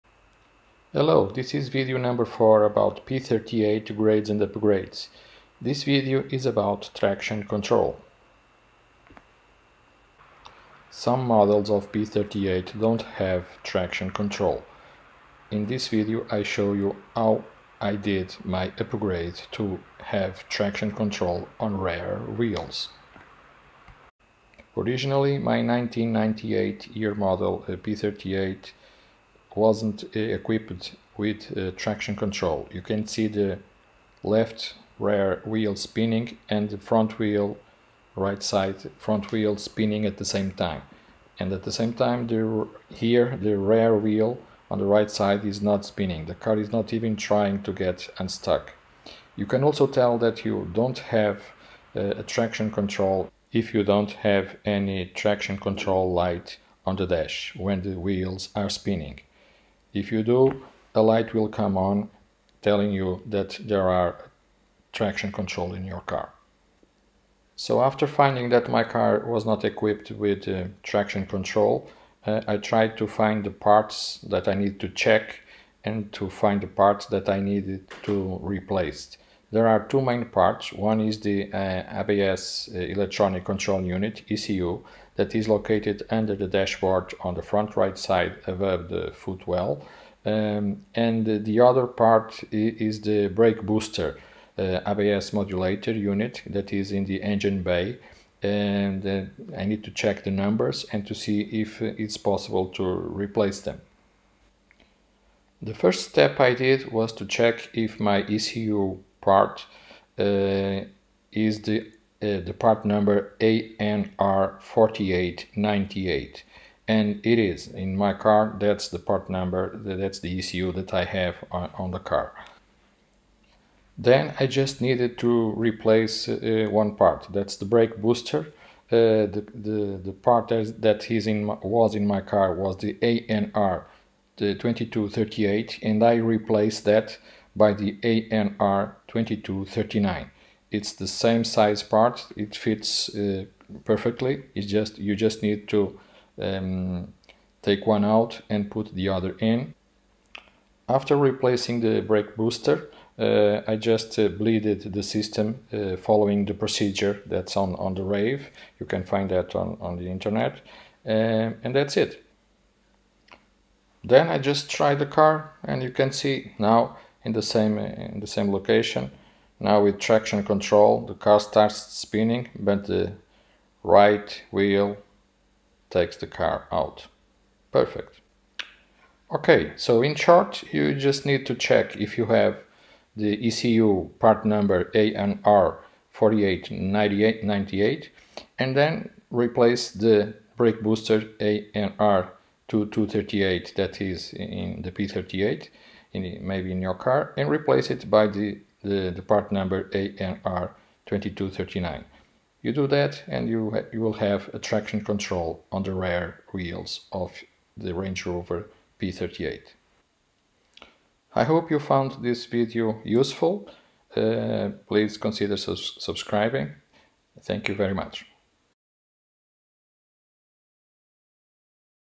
004 P38 Traction Control
Many off-road vehicles aren’t truly off-road because when two wheels from opposite sides are spinning the car stops and doesn’t have momentum to go forward, because the other two wheels don’t get any power. A car with Traction Control (TC) detects this situation and “brakes” the spinning wheels in order to shift power to the other wheels and these get the car moving forward.
Range Rover P38 late models have 4 wheel TC and previous models may have optional rear traction control or no TC at all.
My approach was to replace the parts needed to provide at least rear TC. My YM 1998 P38 don’t have TC because it has the ABS breaking module part no. ANR2238. Replacing this with ABS module ANR 2239 will fix this as long as the ECU module is also the right one.
To have the rear wheels TC option available (up to YM 1999) you must have: The ECU part no. ABS ECU Wabco ANR4898; The Brake Booster part no. ANR2239 (and not ANR2238).